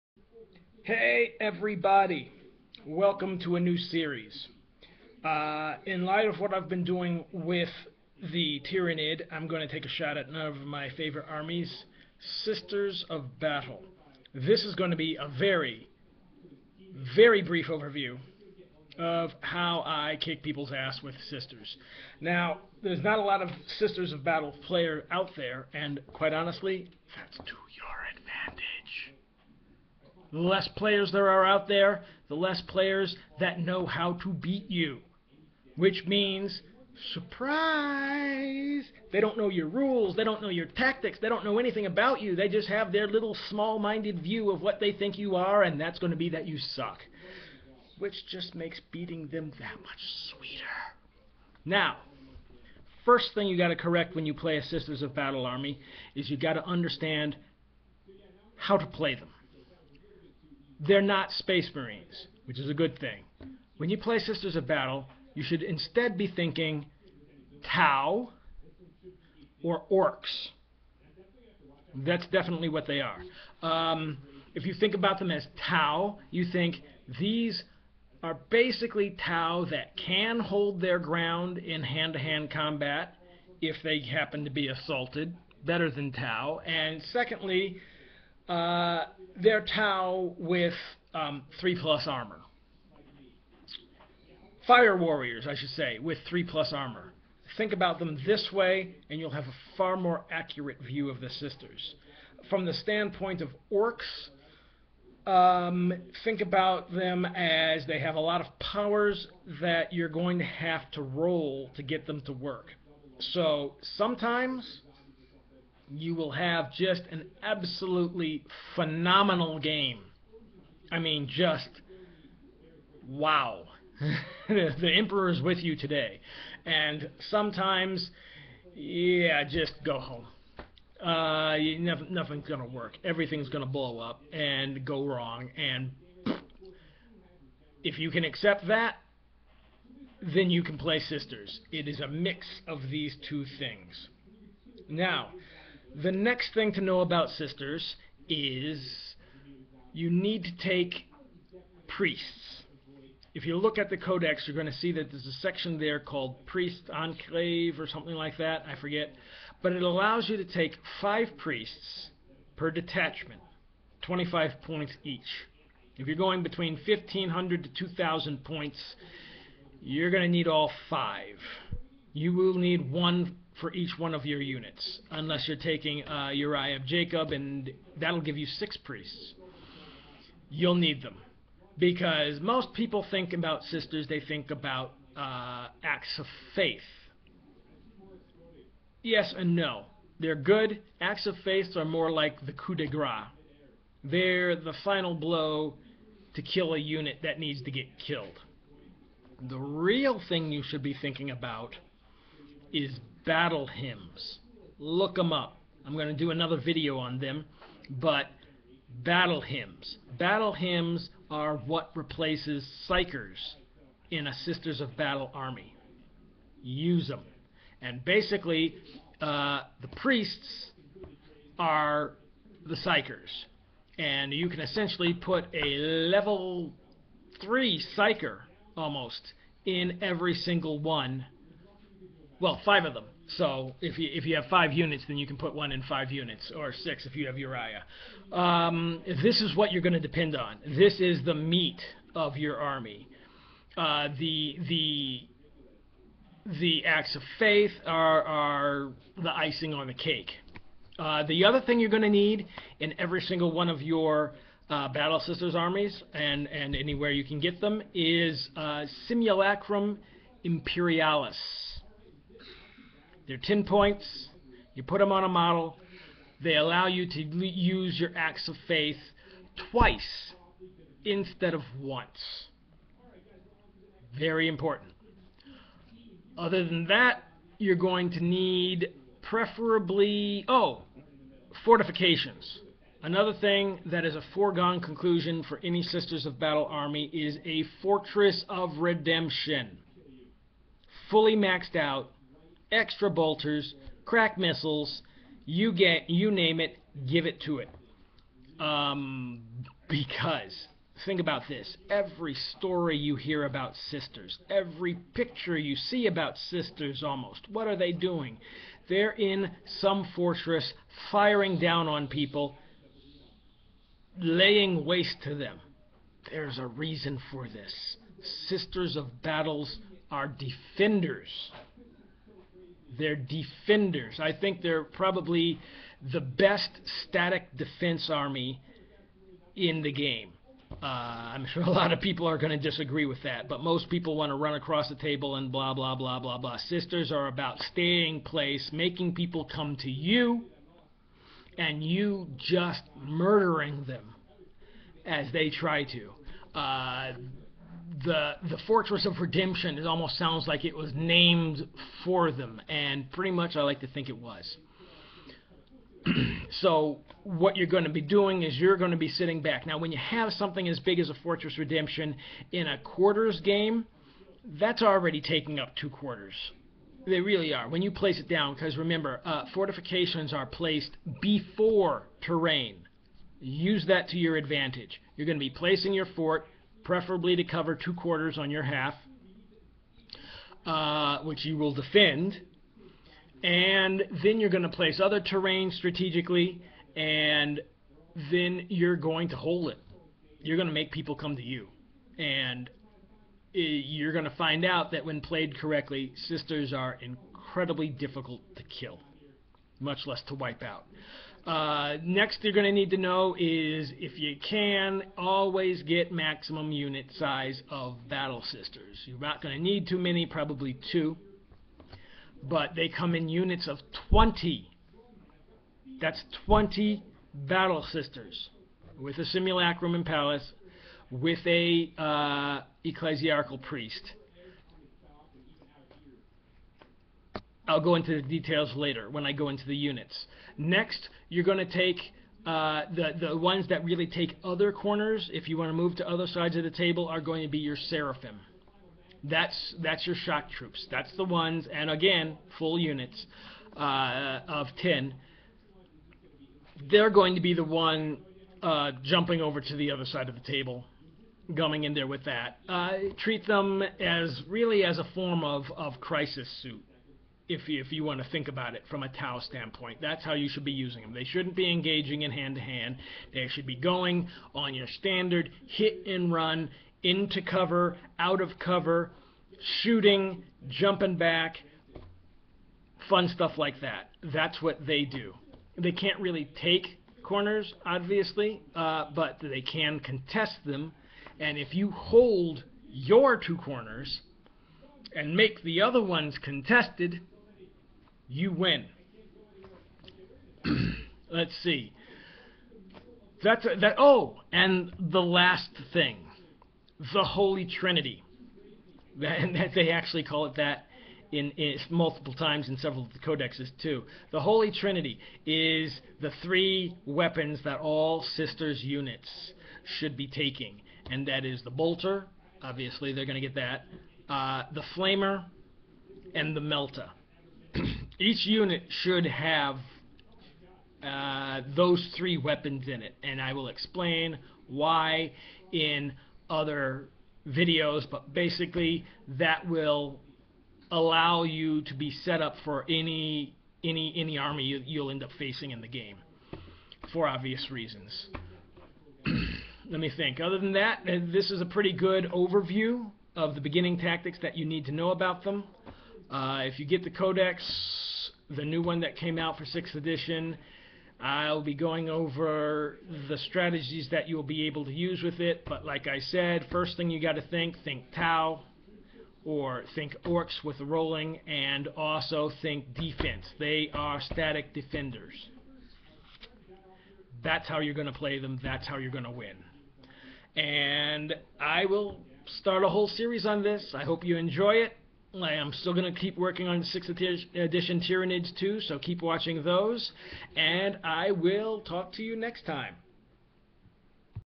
SISTERS OF BATTLE TACTICS OVERVIEW - Warhammer 40k
This is an introduction to Sisters of Battle and how to play them as a competitive army. Like the Tyranids many people say they are impossible to play, I am here to prove them wrong.
    In the end the most deadly weapon in the game is your SKILLZ, not a codex.

Here is the link to the 6th Ed Adepta Sororitas Codex